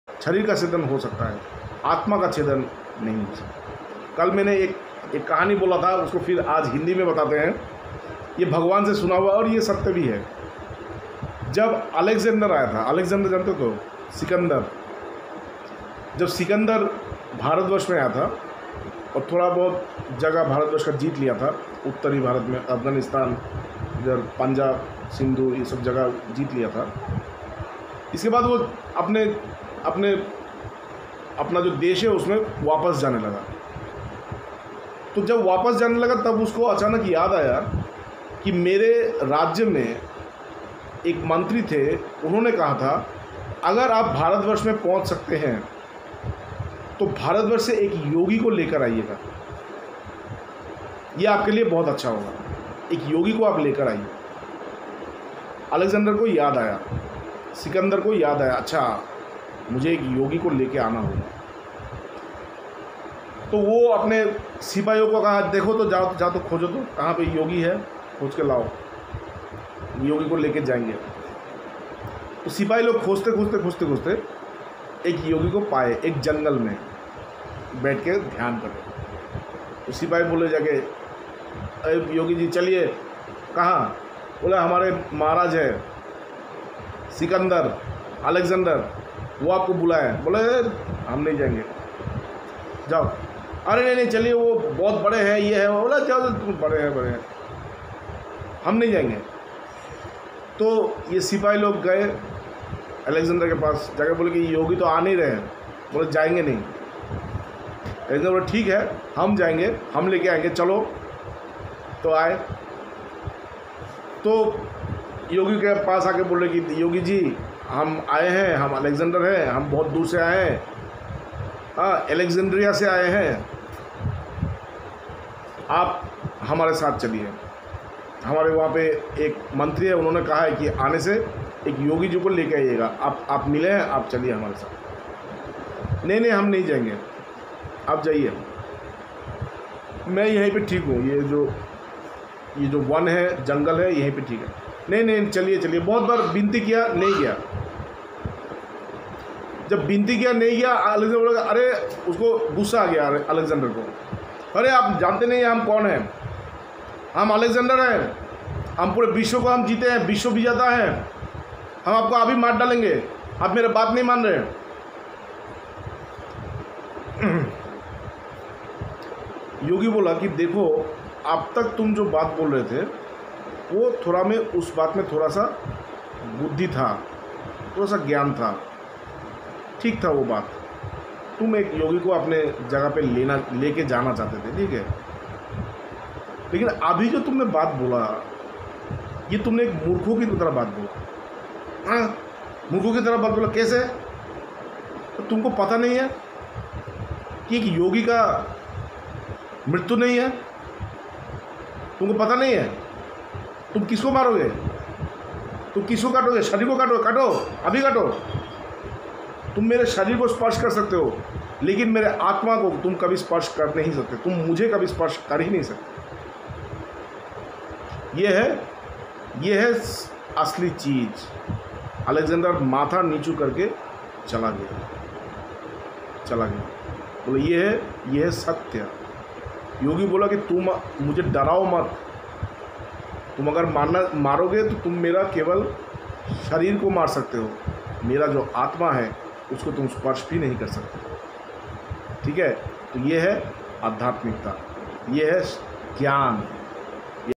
शिकंदर और योगी की कहानी। बातों बातों में श्रीमद्भगवद्गीता ।
लॉकडाउन की स्थिति में श्रीमद्भगवद्गीता पर व्याख्यान के कुछ अंश।